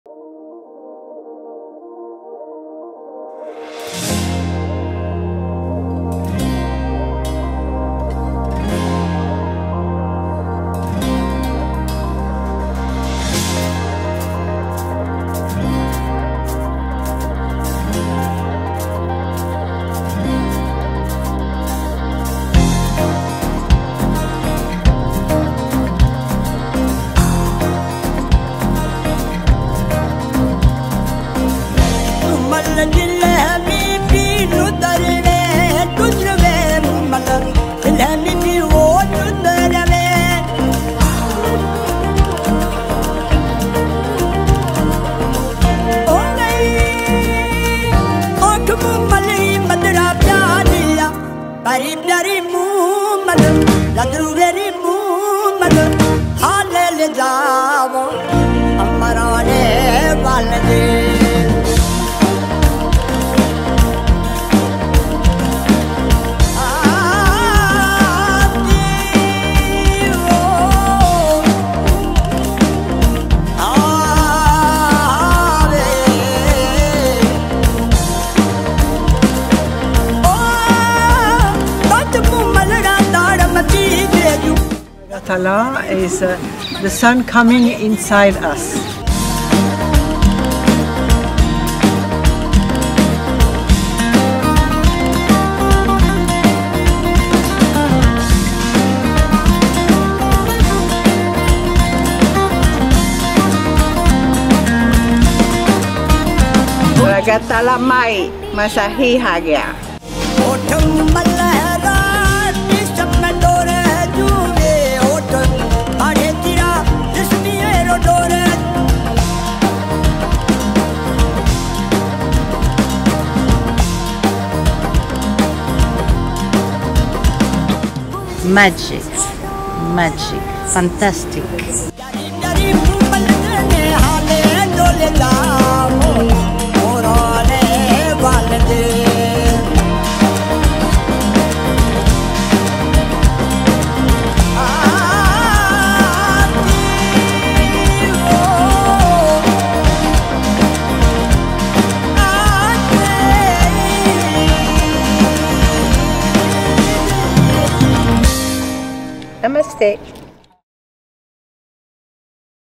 Raaga Taala Retreat 2025: Highlights from Bijolai Palace | Creative Karma Overview
The wait is over! 🎉 Our overview video of the 8th edition of the Raaga Taala Retreat is now live. Relive the magical moments from 16-23 February 2025 at the stunning Bijolai Palace, Jodhpur. Dive into the harmony and creativity that made this retreat truly unforgettable.